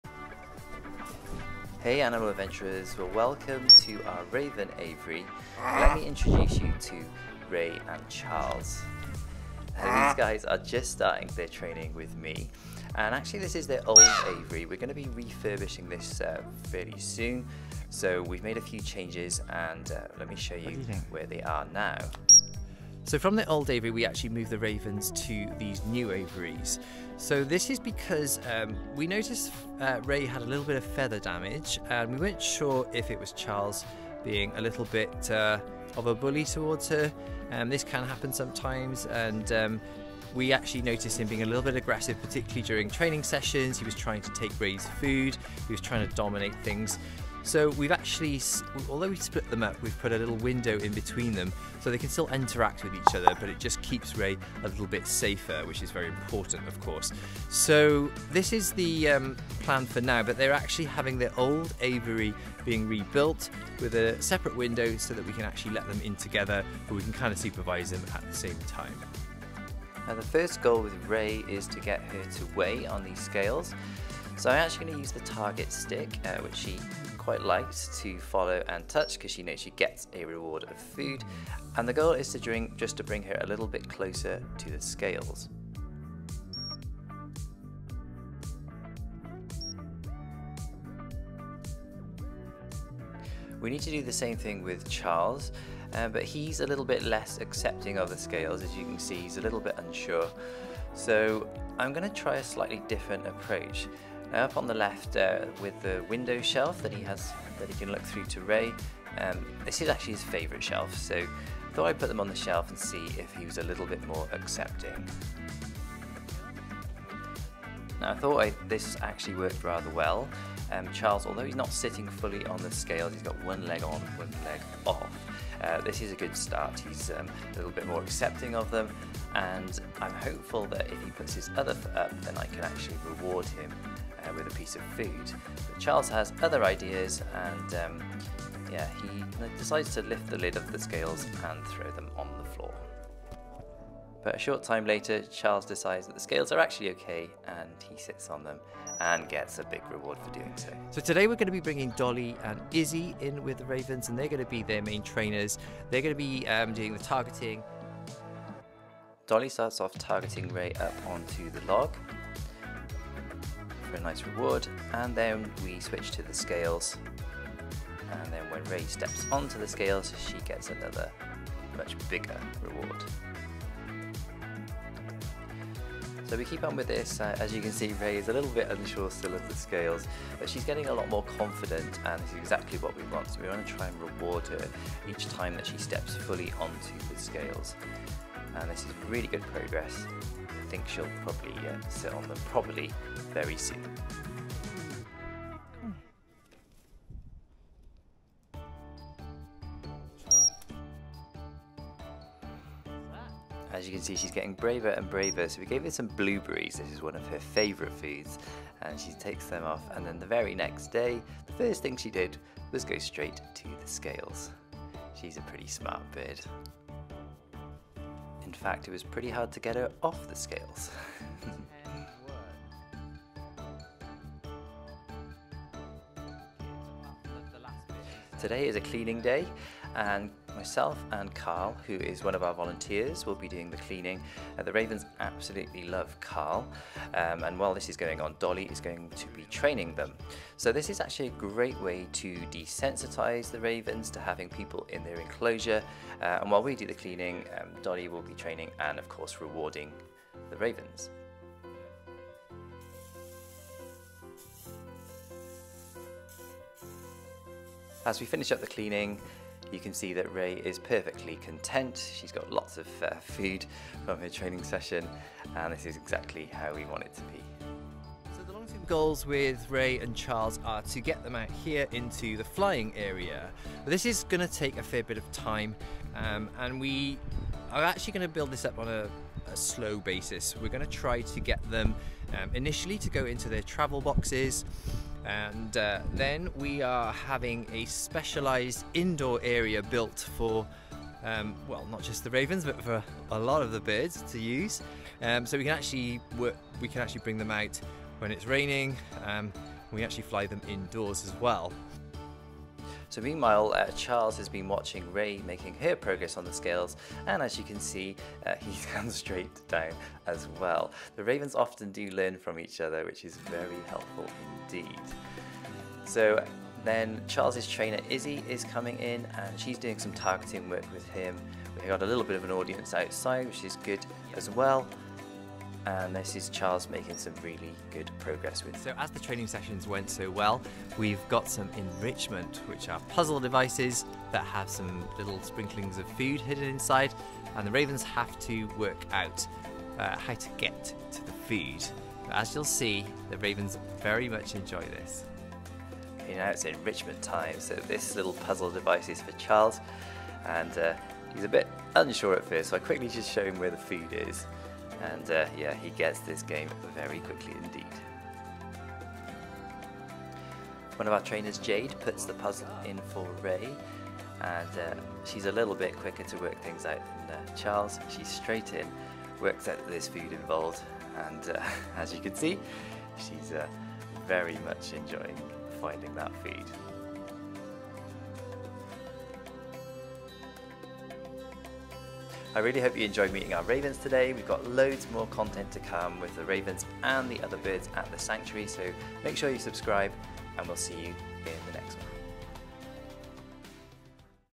Raven Training (They Love it!)
Meet our two playful Ravens, Rey and Charles!

These birds are not only cute but also full of mischief. Watch as we give them puzzles for enrichment and training sessions to keep them mentally stimulated.

From playful antics to clever tricks, you won't want to miss a moment of the fun with Rey and Charles.

⌚ Video Content

0:00 - Meet Rey & Charles
1:10 - First day of training Rey
1:35 - Weighing with Charles
2:39 - Trainer Dolly
3:32 - Rey Success!
3:48 - Cleaning Day
4:29 - Long term goals
5:04 - Charles' Success!
5:16 - Trainer Izzy
5:33 - Enrichment!